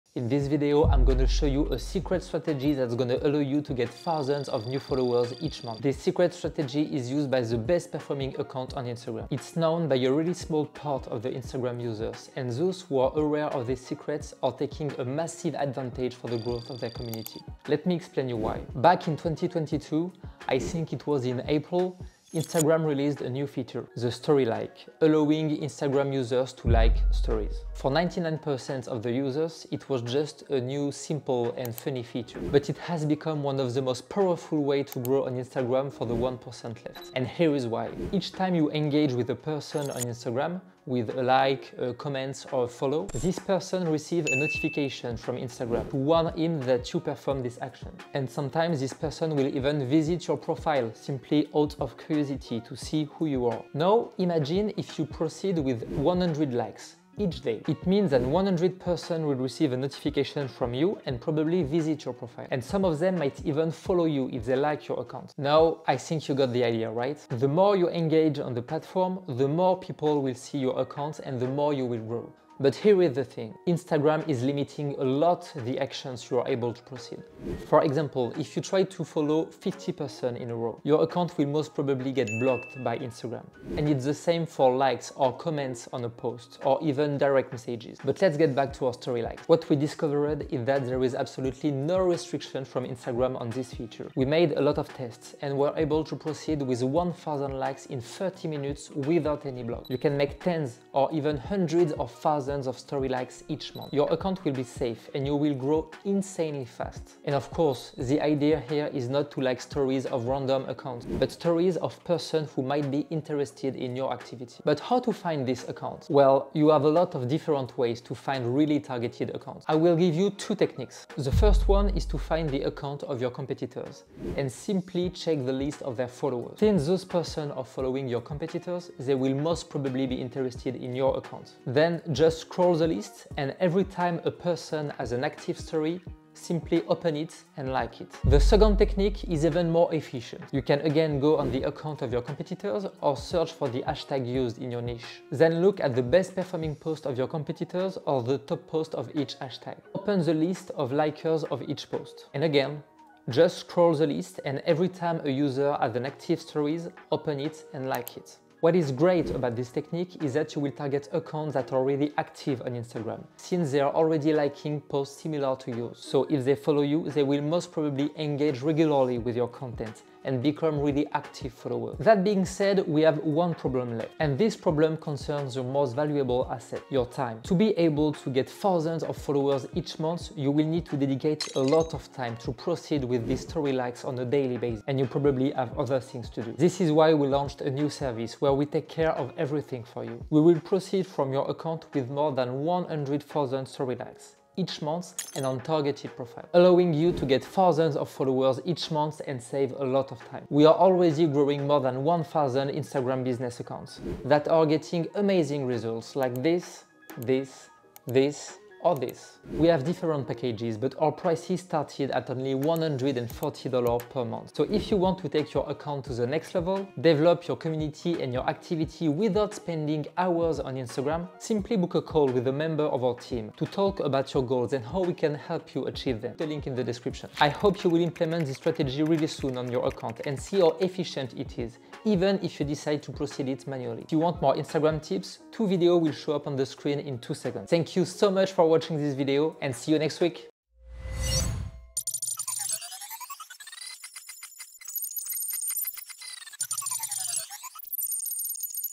Secret Hack to get Thousands of Followers on Instagram (you won't believe it)
This secret technic is used by the best-performing Instagram accounts.
It's known by a tiny part of Instagram users, and those aware of this secret are taking a massive advantage to grow their Instagram accounts.